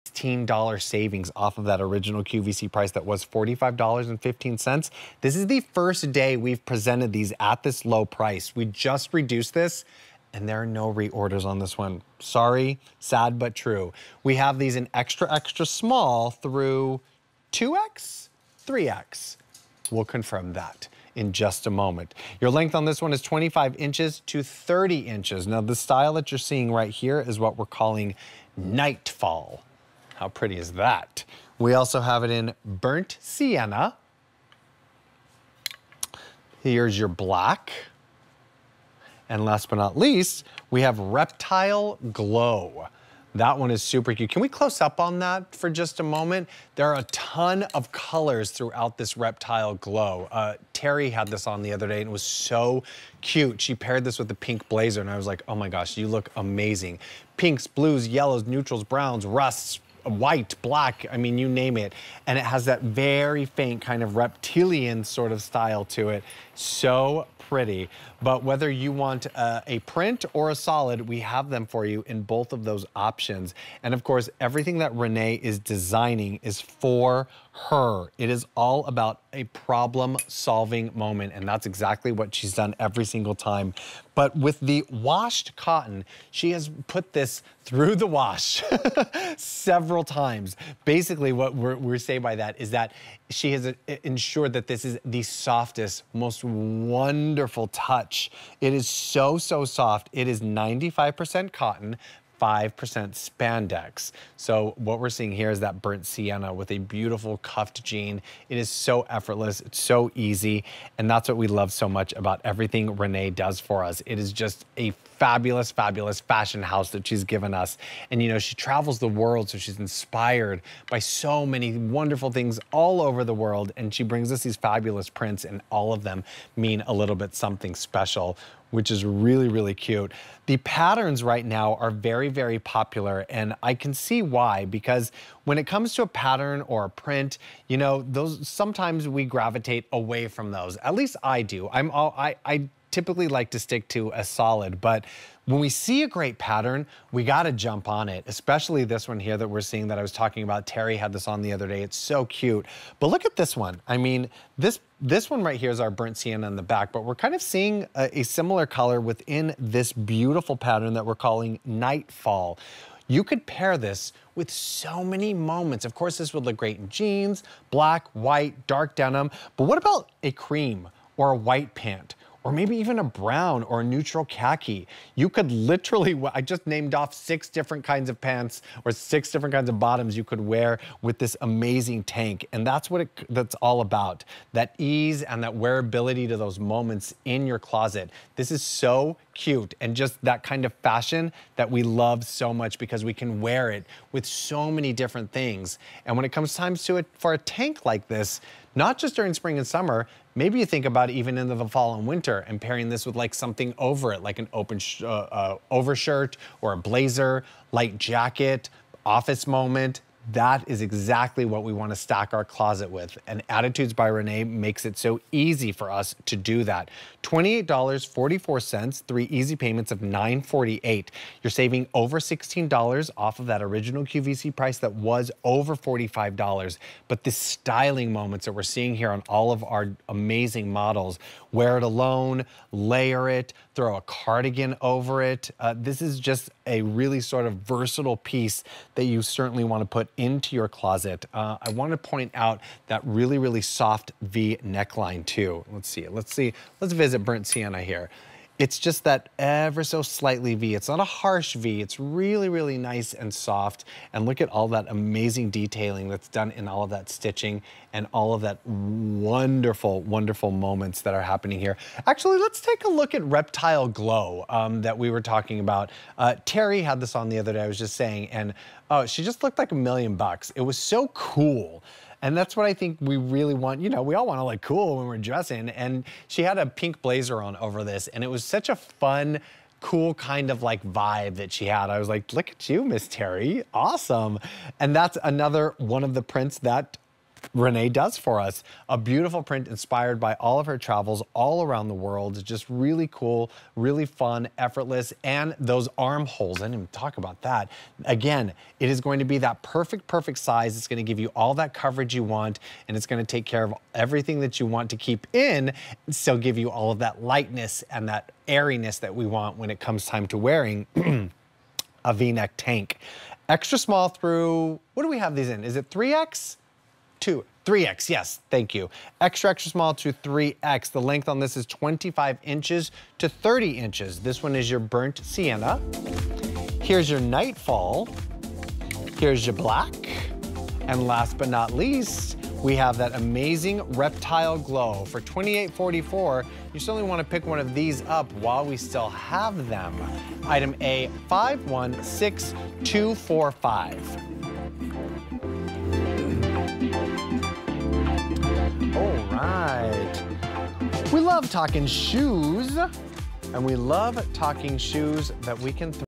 Attitudes by Renee Washed Cotton Jersey V-Neck Tank on QVC
Attitudes by Renee Washed Cotton Jersey V-Neck Tank

Sport this dashing V-neck tank to all those fancy summertime soirees you've got sprinkled all over your calendar. Warm-weather chicness? Yes, please. From Attitudes by Renee&reg;.